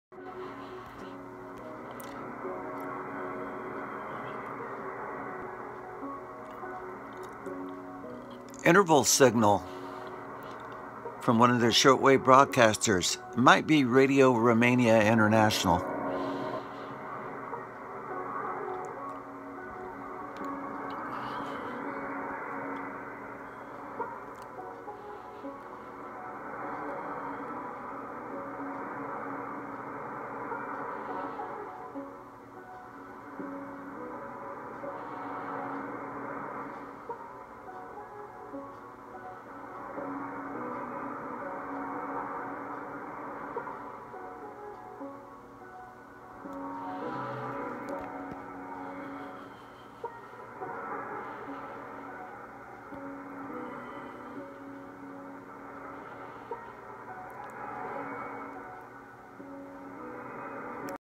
Interval Signal Radio Exterior De Espana
I say radio Romania in the video but I meant to say radio España international a.k.a. radio Spain international
Shortwave radio station interval signal on 9690. The radio is my DX 394 with a 18 foot whip antenna vertical